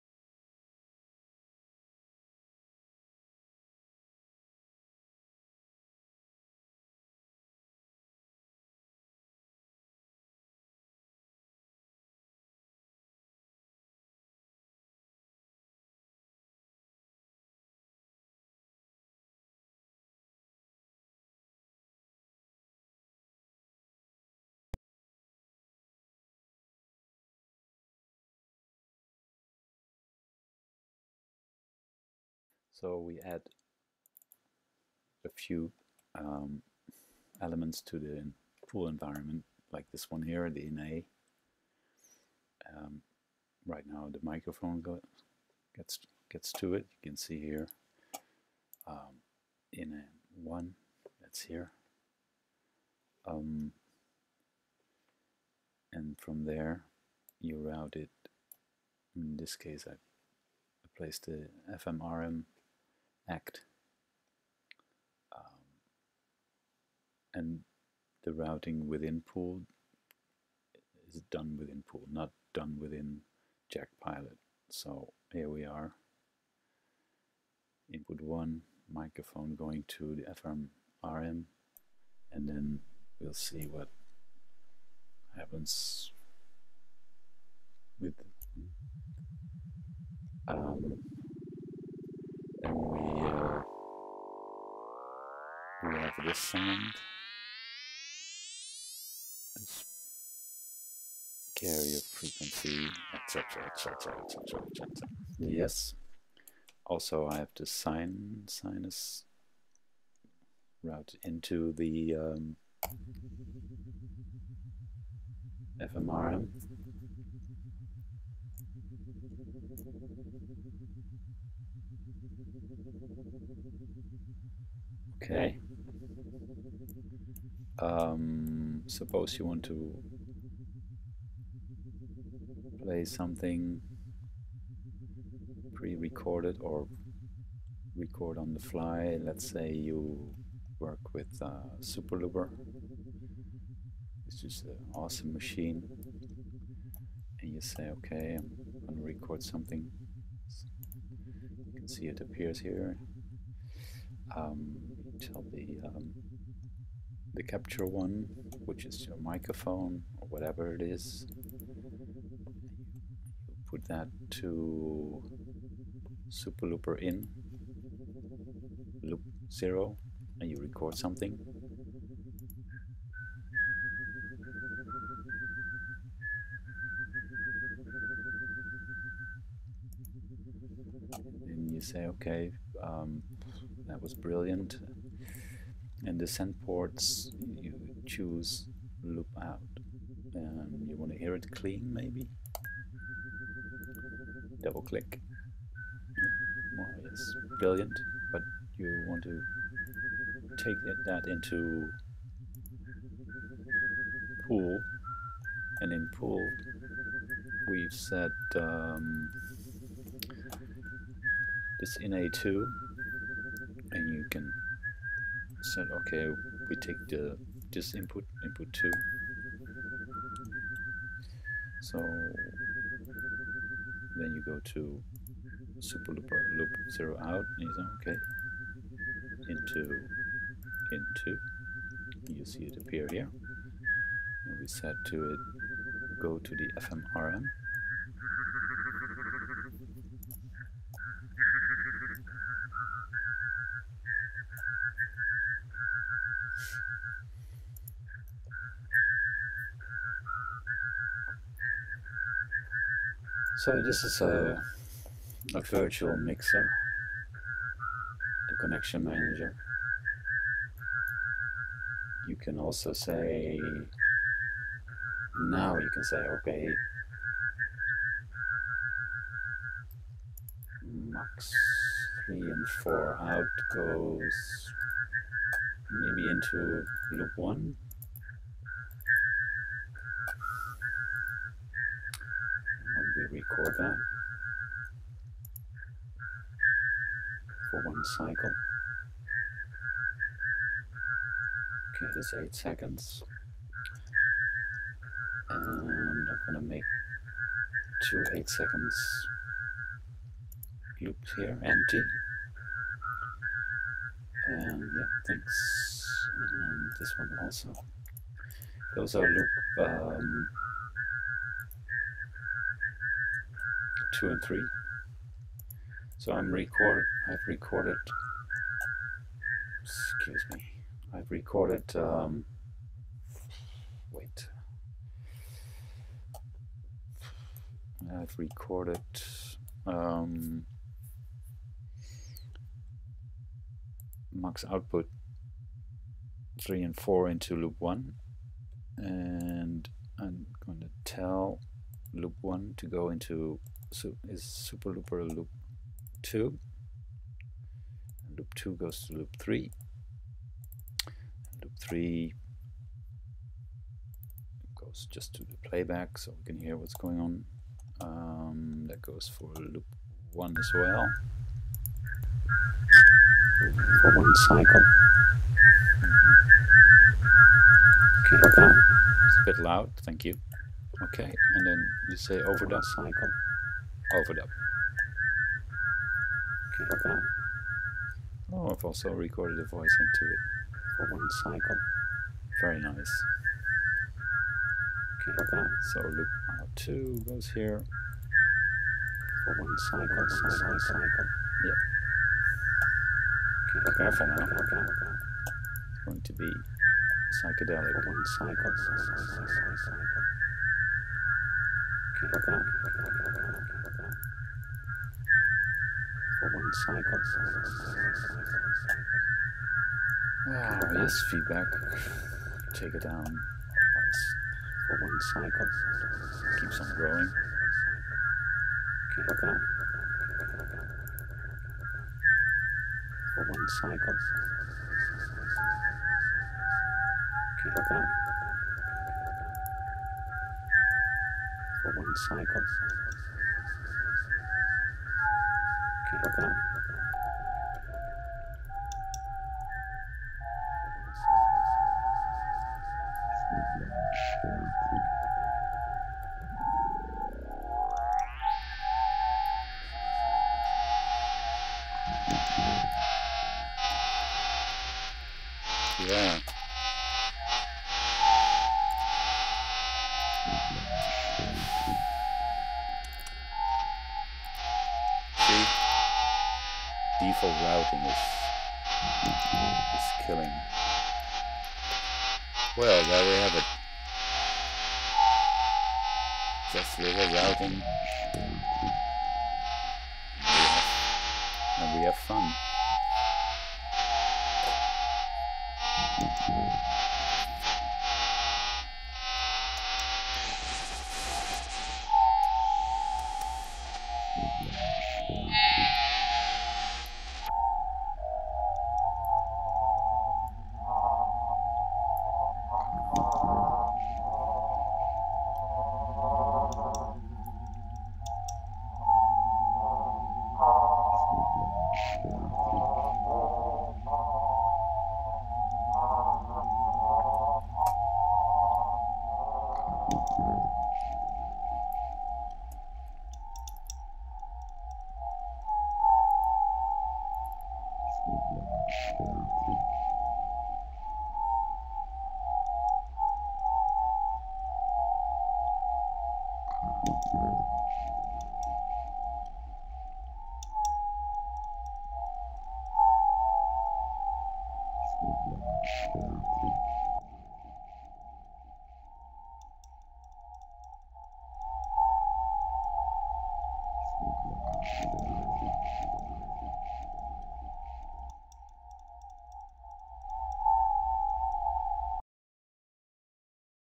PpoolL Audio Routing & Recording pt.2 w/ Jackrouter & Sooperlooper
PpoolL Basic Audio Routing & Recording w/ Jackrouter Sooperlooper. its all free & works!  How to route audio from anywhere to everywhere with a view clicks, and how to record into your Ppooll actions. Part Two

routing expanding a bit. With Ina in ppooll and a fmrm @ct and sooperlooper instant karma.